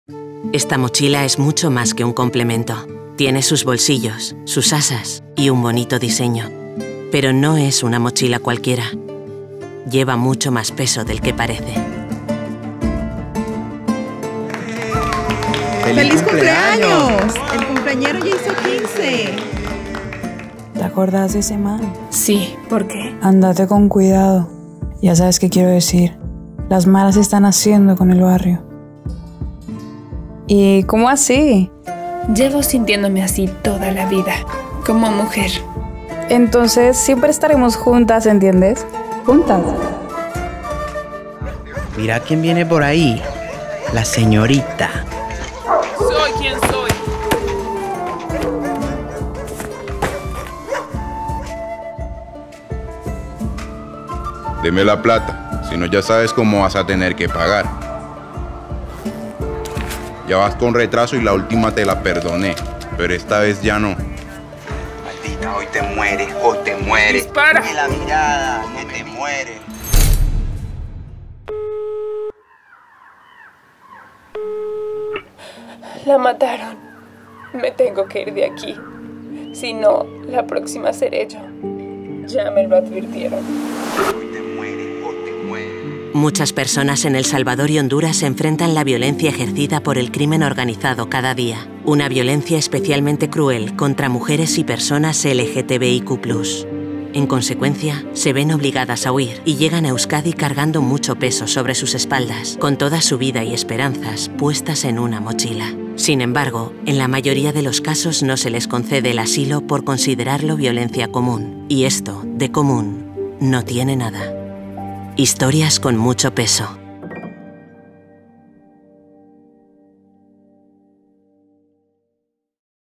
Historias con mucho peso
Muchas personas de El Salvador y Honduras llegan a Euskadi huyendo del Crimen Organizado (maras, narcotráfico, agentes estatales y paraestatales). Las mujeres y personas LGTBIQ+ son especialmente perseguidas por el carácter altamente machista de estos grupos delincuenciales.

En sus mochilas traen historias cargadas de violencia, pero también de resistencia, lucha esperanza y vivencias que compartir con la sociedad que les acoge. Son HistoriasConMuchoPeso.